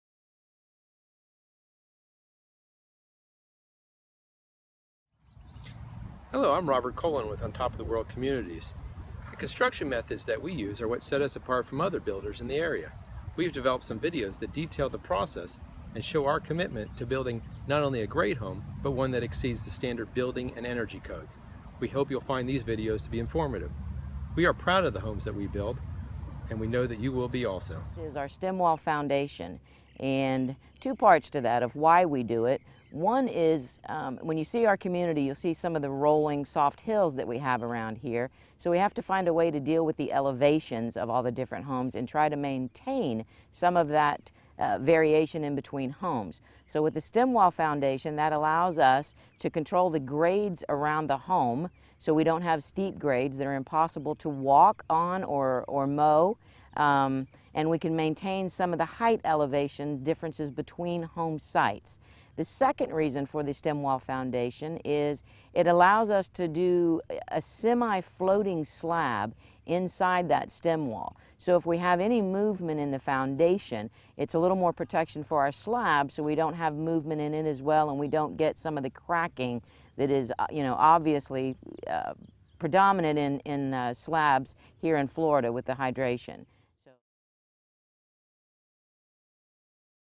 Stem Walls - Construction at On Top of the World Communities
On Top of the World Communities in Ocala, Fl explains the use of a stem wall footer and the benefits this accomplishes. In this construction series, OTOW discusses building methods utilized at On Top of the World that set us apart from other builders in the area. Simply a better built home.